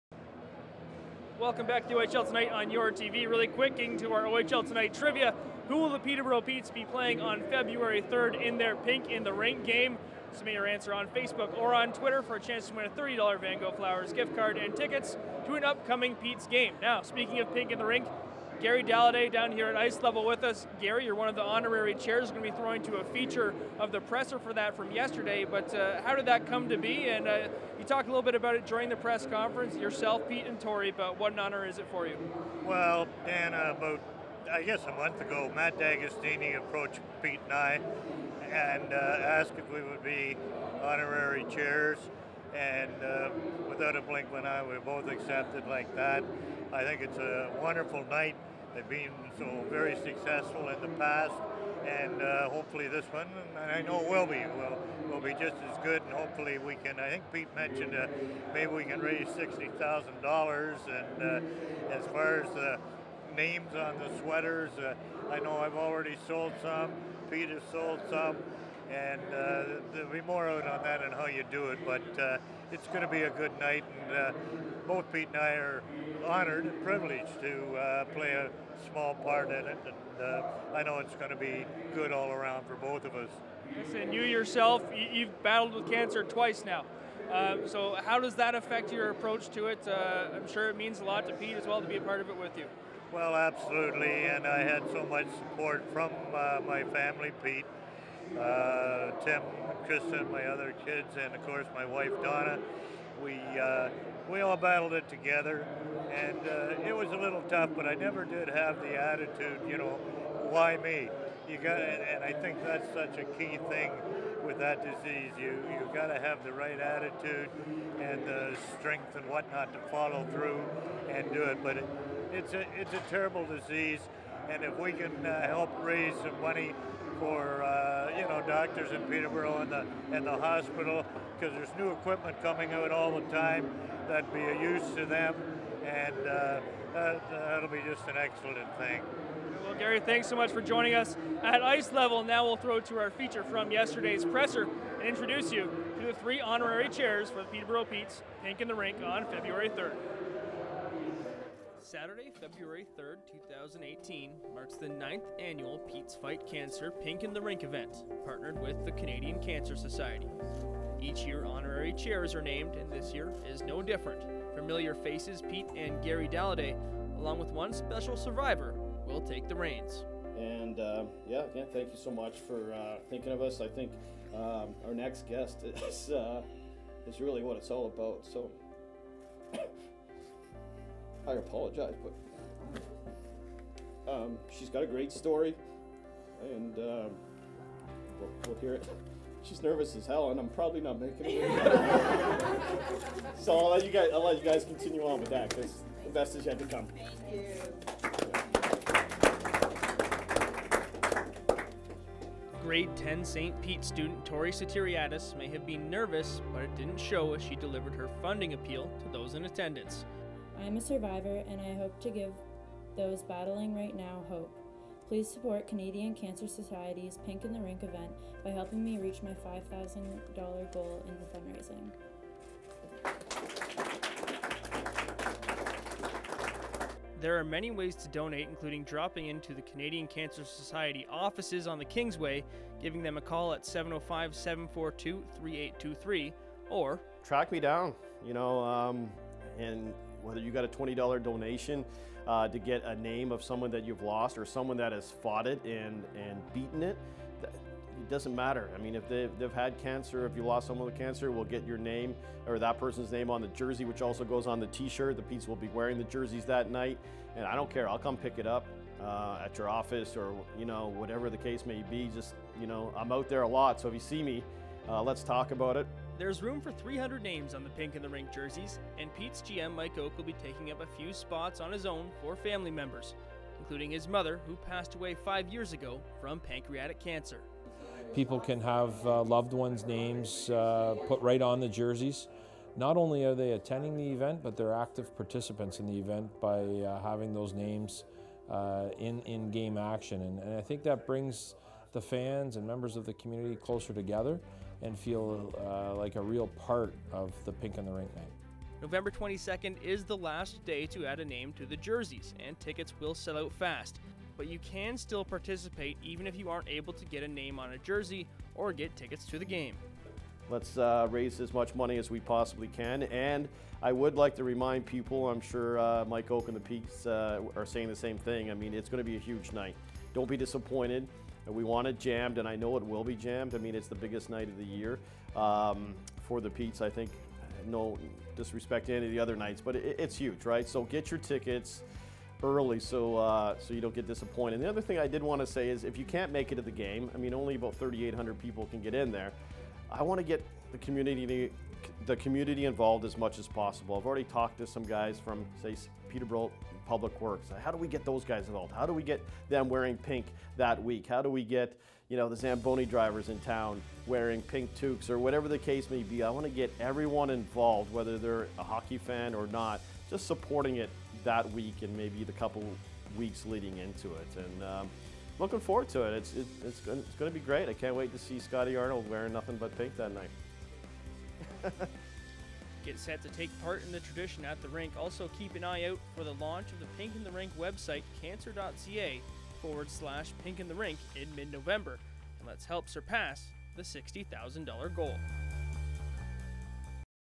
Petes: Pink in the Rink, Honourary Chairs Announced
Tori Sotiriadis, Pete Dalliday and Gary Dalliday will be the honourary chairs for the 9th annual Pink in the Rink game.